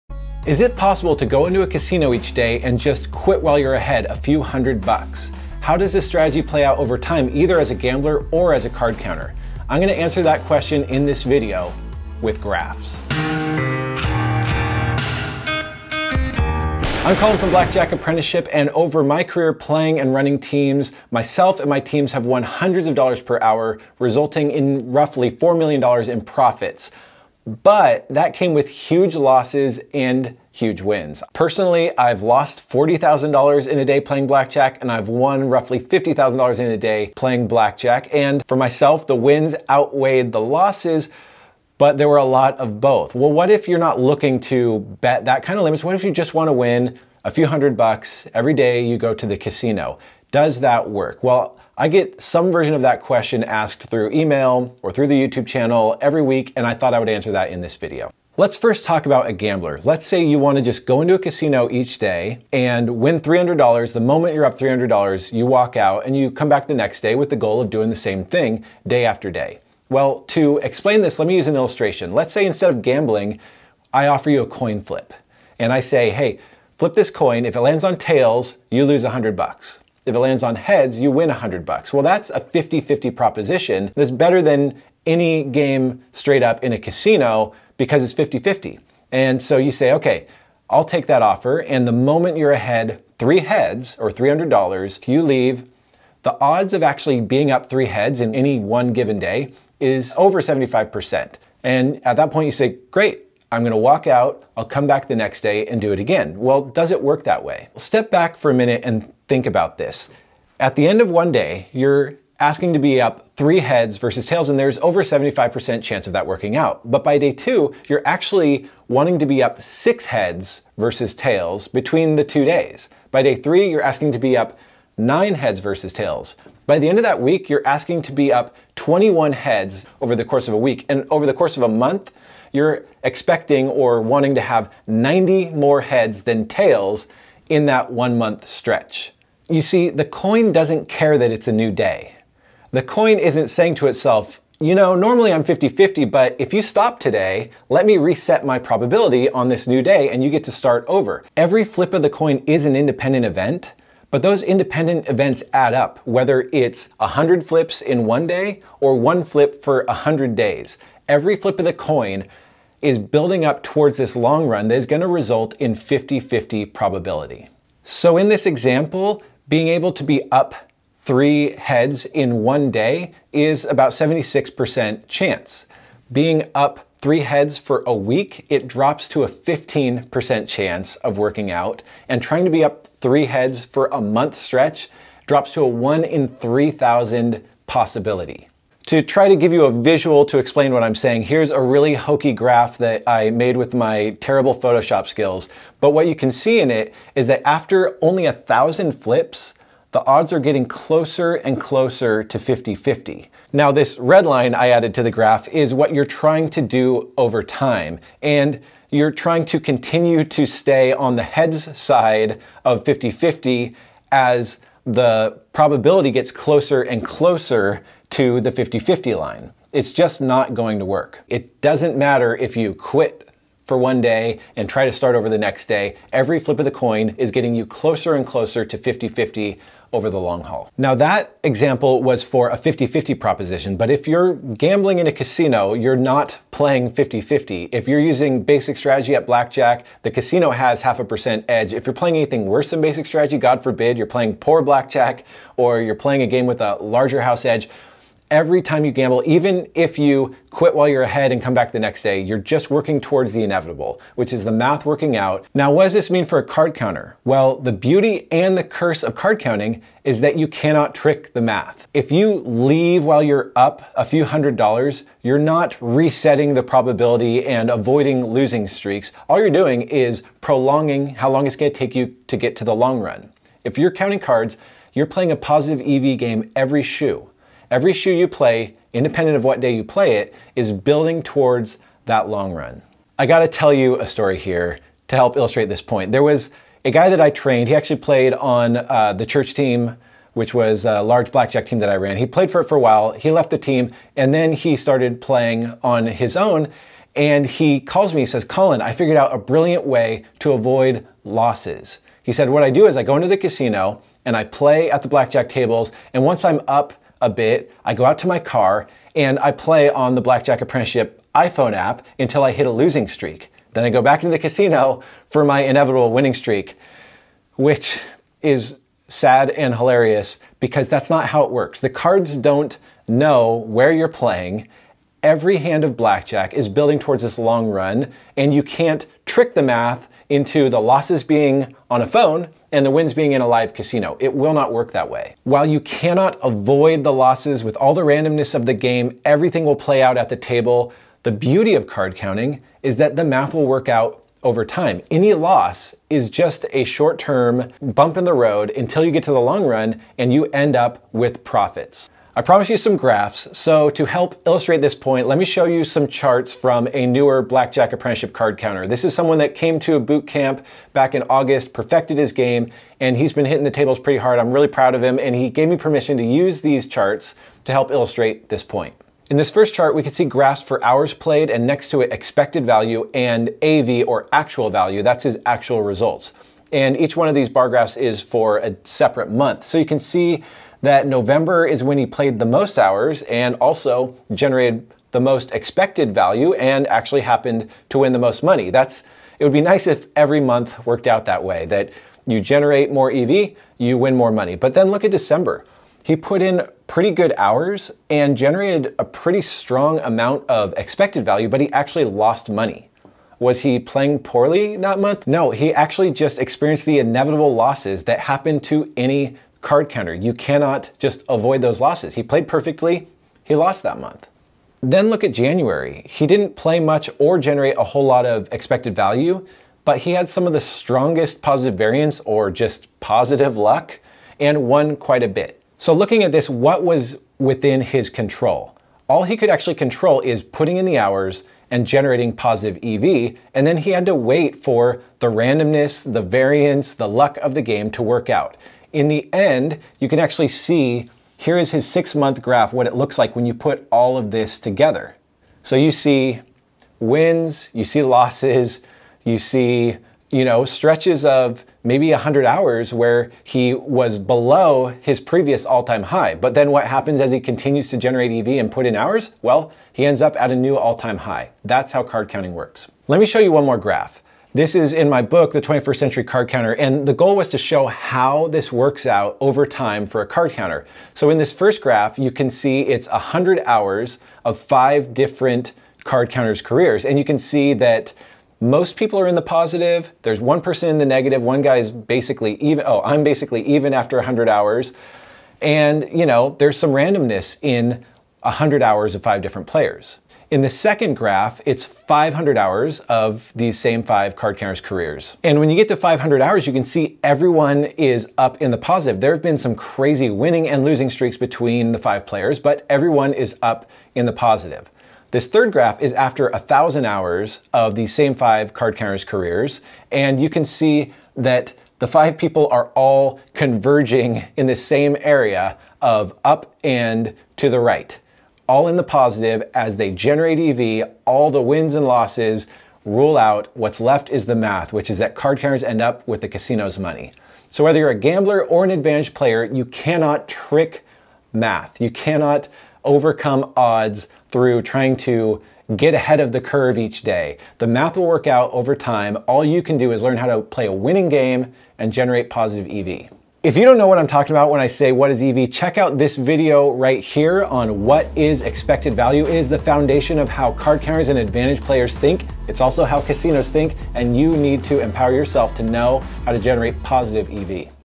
"Win a Few Hundred Bucks a Day" Blackjack Strategy: Does It Work?
You’re not greedy, you just want to win a little and not get backed off.  What are the odds you can walk out of the casino with a few hundred bucks tonight?

0:00 - Introduction: Can You Just Quit While You're Ahead?
0:59 - Coin Flip Analogy: How Probability Adds Up Over Time
2:52 - Gambler Strategy Over Multiple Days
4:17 - Card Counter Strategy: Why You Can't Trick the Math
5:52 - Card Counter Graphs: Wins, Losses, and EV
7:46 - Final Takeaway: The Math Always Wins